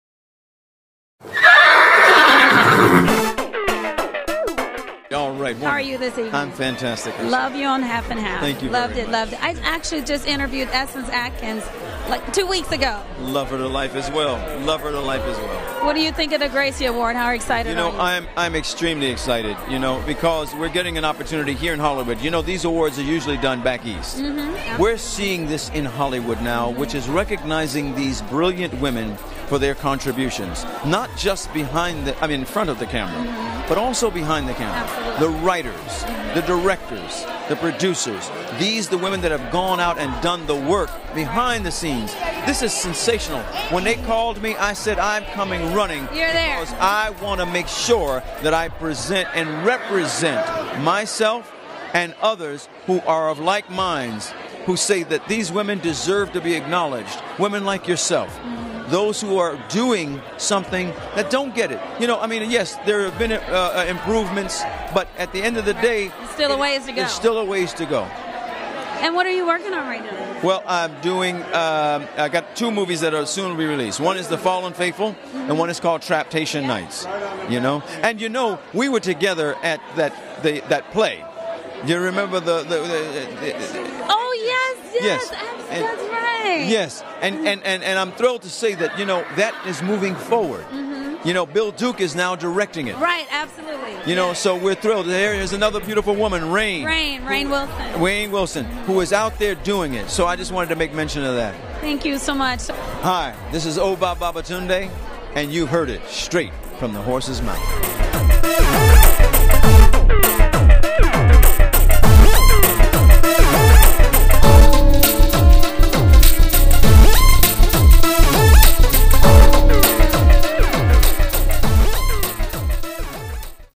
Obba Babatunde' Talks about Women in Hollywood
SFTHM Host, Ursula Joyner covers the Gracie Awards